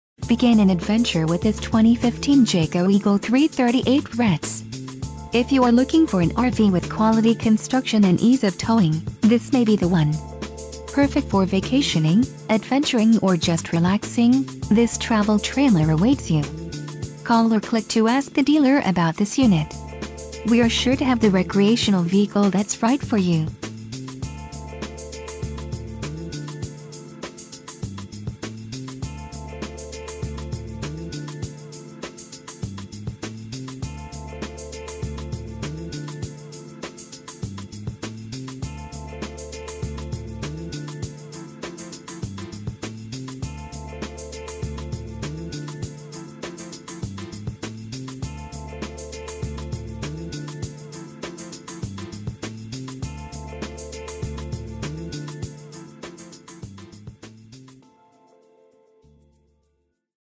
2015 Jayco Eagle 338RETS - Fretz RV - Souderton, PA 18964
For Sale in Souderton, PA 18964

2015 JAYCO EAGLE 338RETS TRAVEL TRAILERWE CAN ONLY ADVERTISE MSRP, CALL FOR SALE PRICE Comes with Jayco&apos;s Customer Value Package, Premier Package, electric tongue jack, ledge stone fireplace w/40&quot; TV, hard surface countertop, hide-a-bed, freestanding table w/4 chairs, king bed, exterior bumper mount grill and roof ladder.FREE Camping membership with every purchase, click here to learn more.Like us on Facebook!Length Overall (LOA): 396,